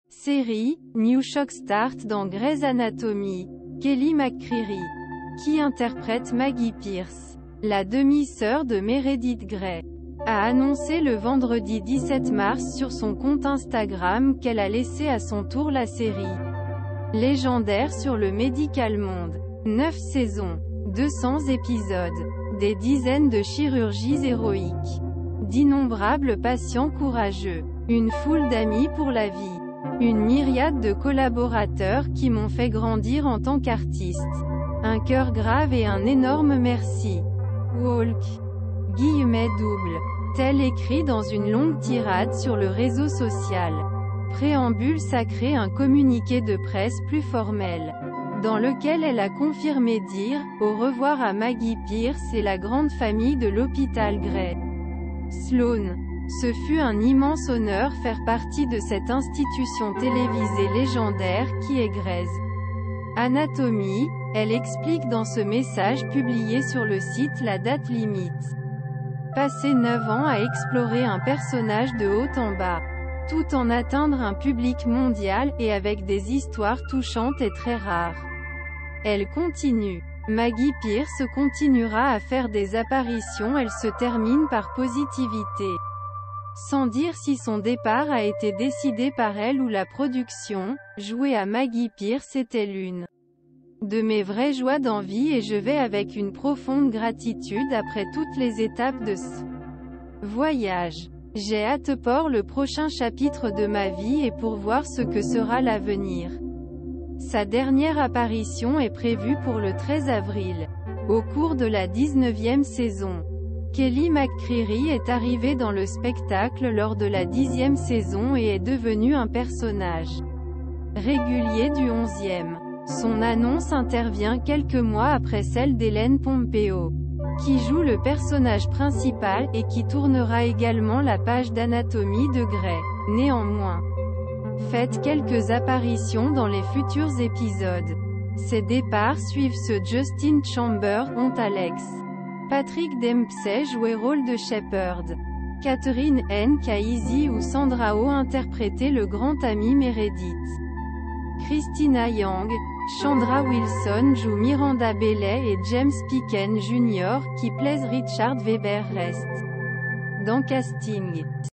Kelly McCreary quitte « Grey’s Anatomy » à son tour
Série - New Shock Start dans Grey's Anatomy.Kelly McCreary, qui interprète Maggie Pierce, la demi-sœur de Meredith Gray , a annoncé le vendredi 17 mars sur son compte Instagram qu'elle a laissé à son tour la série légendaire sur le médicalmonde. "Neuf saisons, 200 épisodes, des dizaines de chirurgies héroïques, d'innombrables patients courageux , une foule d'amis pour la vie, une myriade de collaborateurs qui m'ont fait grandir en tant qu'artiste, un cœur grave et un énorme merci.Walk! ", t-elle écrit dans une longue tirade sur le réseau social. Préambule sacré un communiqué de presse plus formel, dans lequel elle a confirmé dire "au revoir à Maggie Pierce et la grande famille de l'hôpital Gray Sloan". "Ce fut un immense honneur faire partie de cette institution télévisée légendaire qui est Grey's Anatomy,"Elle explique dans ce message publié sur le site la date limite. "Passer neuf ans à explorer un personnage de haut en bas, tout en atteindre un public mondial et avec des histoires touchantes est très rare. "Elle continue. Maggie Pierce continuera à faire des apparitions Elle se termine par positivité, sans dire si son départ a été décidé par elle ou la production "Jouer à Maggie Pierce était l'une de mes vraies joies dans vie et je vais avec une profonde gratitude après toutes les étapes de ce voyage. J'ai hâtepour le prochain chapitre de ma vie et pour voir ce que sera l'avenir. Sa dernière apparition est prévue pour le 13 avril, au cours de la 19e saison. Kelly McCreary est arrivée dans le spectacle lors de la 10e saison et est devenue un personnage régulier du 11e. Son annonce intervient quelques mois après celle d'Ellen Pompeo, qui joue le personnage principal et qui tournera également la page d'anatomie de Gray.Néanmoins, faites quelques apparitions dans les futurs épisodes. Ces départs suivent ceux Justin Chambers ont Alex, Patrick Dempsey joué rôle de Shepherd, Katherine Heigk a Izzie ou Sandra Oh interprété le grand ami Meredith, Cristina Yang.Chandra Wilson joue Miranda Bailey et James Picken Jr quiPlays Richard Webber reste dans casting.